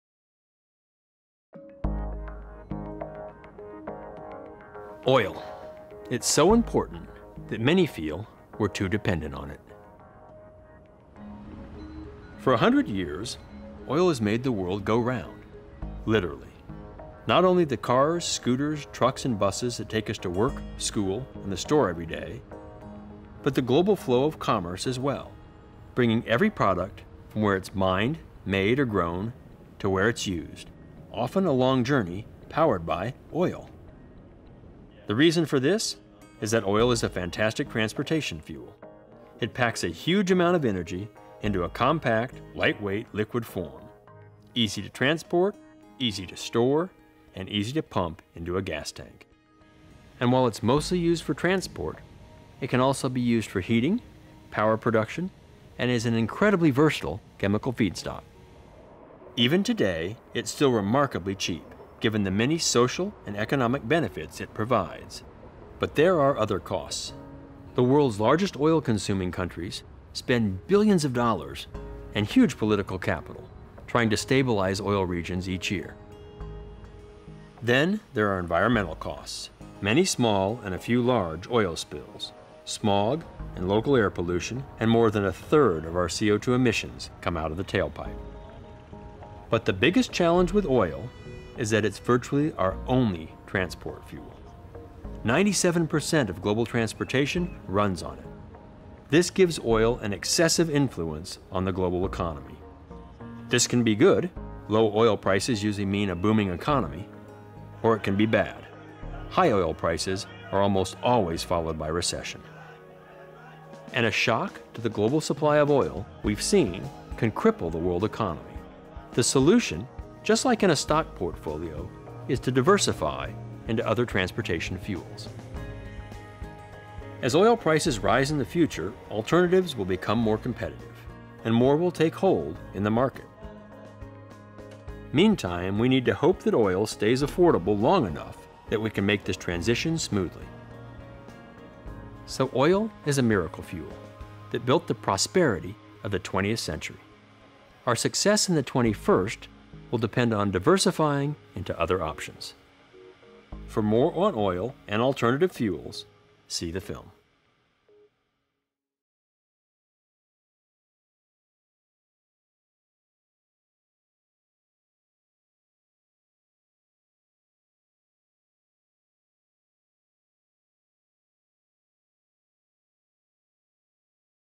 Oil 101
"97% of global transportation runs on oil." Although oil's ability to pack energy into a compact liquid form makes it highly accessible, it accounts for the majority of our carbon dioxide emissions and has caused devastating spills. (2012)